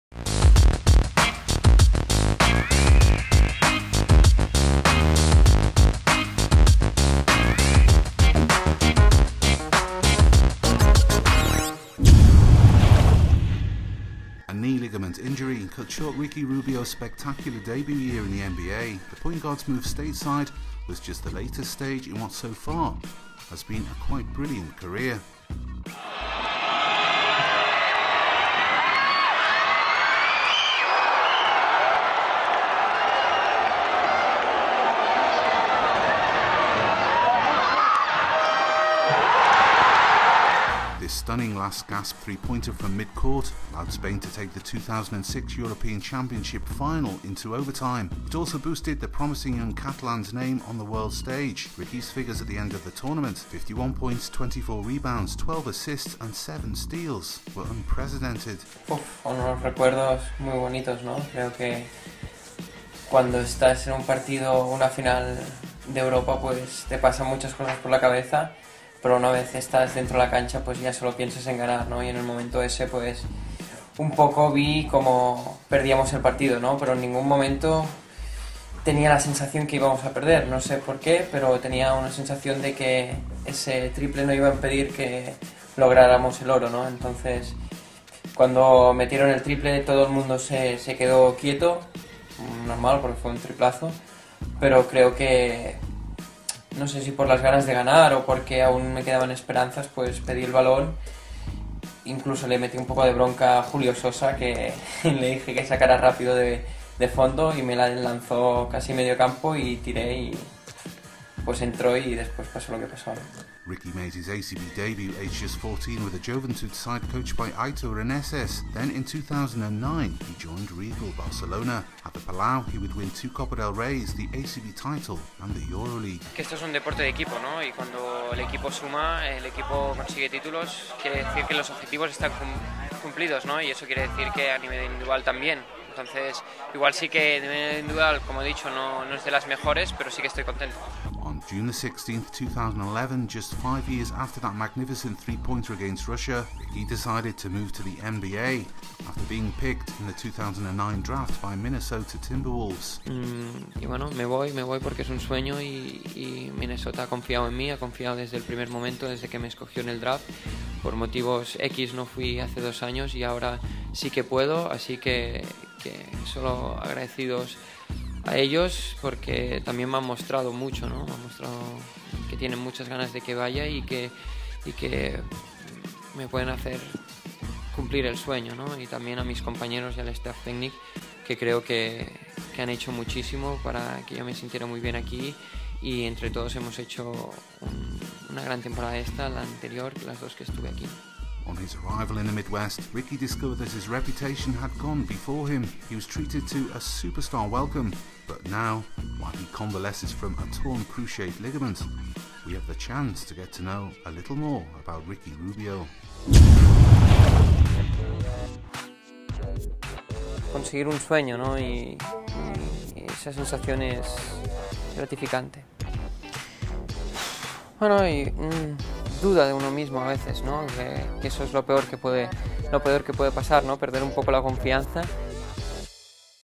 Ricky Rubio's story (I): METEORIC RISE
We start in Basket4us a four-part serial about the career of Ricky Rubio. In this chapter, we review the professional time of the point guard in the Spanish League ACB, to be drafted by Minnesota Timberwolves in the NBA.